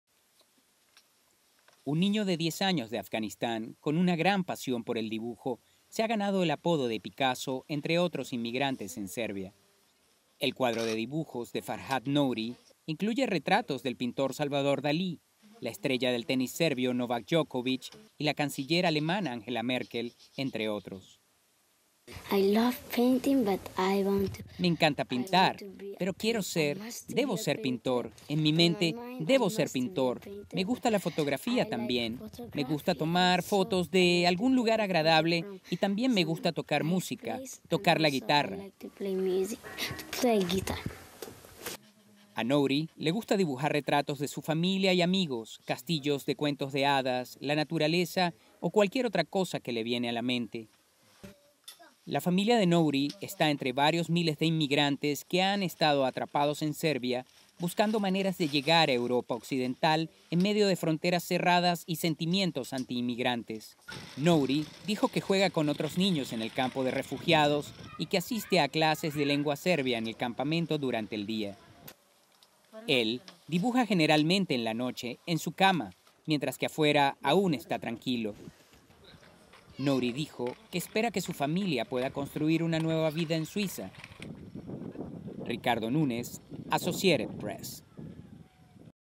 'Pequeño Picasso' afgano en campamento de refugiados serbio
(15 Mar 2017) RESTRICTION SUMMARY:
STORYLINE:
Un niño de 10 años de Afganistán con una pasión por el dibujo se ha ganado el apodo de 'Picasso' entre otros inmigrantes en Serbia.
El cuaderno de dibujos de Farhad Nouri incluye retratos del pintor Salvador Dali, la estrella del tenis serbio Novak Djokovic Y  la canciller alemana Angela Merkel entre otros
Farhad Nouri, migrante de 10 años de Afganistán:
"Me encanta pintar, pero quiero ser, debo ser pintor, en mi mente, debo ser pintor, me gusta la fotografía también, me gusta tomar una foto de (de) algún lugar agradable y también me gusta Tocar música, tocar la guitarra. "
A Nouri también le gusta dibujar retratos de su familia y amigos, castillos de cuento de hadas, la naturaleza o cualquier otra cosa que  le viene a la mente.
La familia Nouri está entre varios miles de inmigrantes que han estado atrapados en Serbia buscando maneras de llegar a Europa occidental en medio de fronteras cerradas y sentimientos anti-migrantes.
 Nouri dijo que juega con otros niños en el campo de refugiados y que asiste a clases de lengua serbia en el campamento durante el día.
 Él dibuja generalmente en la noche, en su cama, mientras que afuera esta aun tranquilo.
Nouri dijo que espera que su familia pueda construir una nueva vida en Suiza.